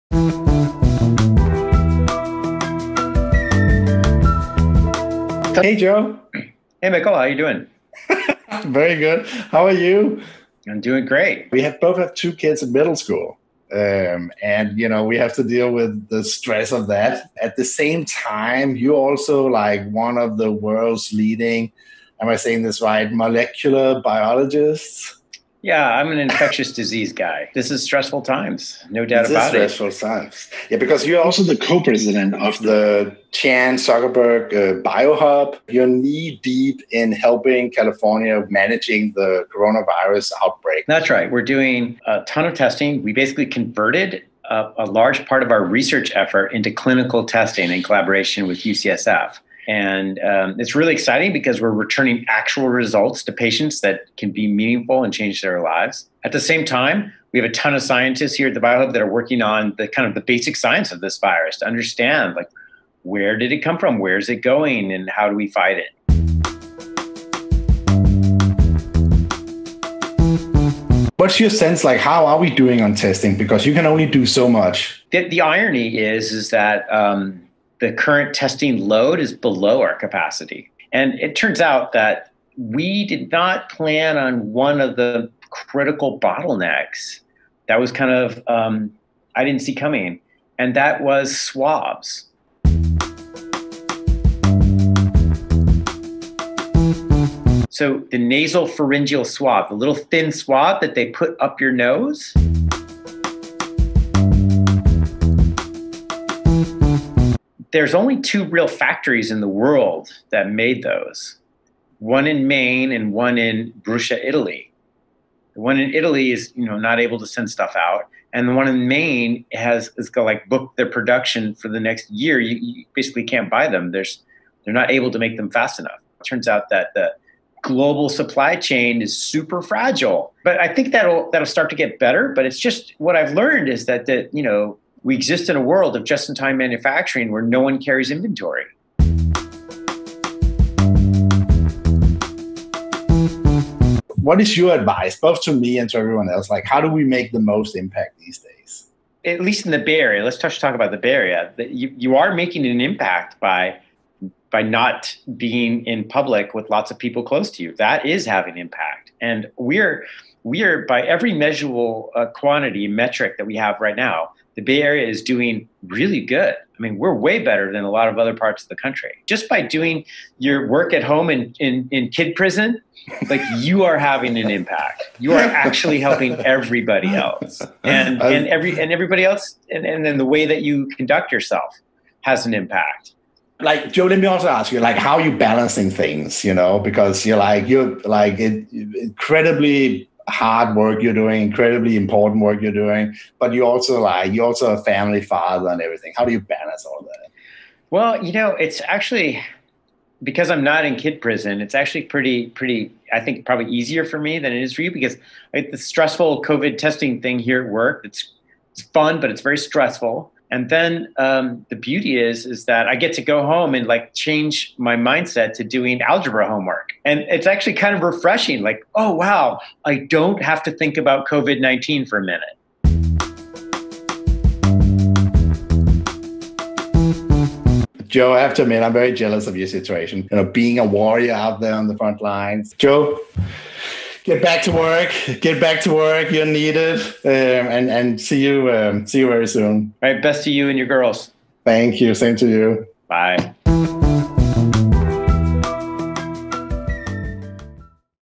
A CEO and an infectious diseases guy meet
Talking swabs, supply-chains, and middle-school kids with Joe DeRisi, Co-President of the Chan Zuckerburg Biohub.

Shared Screens with Mikkel Svane features the Zendesk CEO catching up with his friends and leaders in business and the community.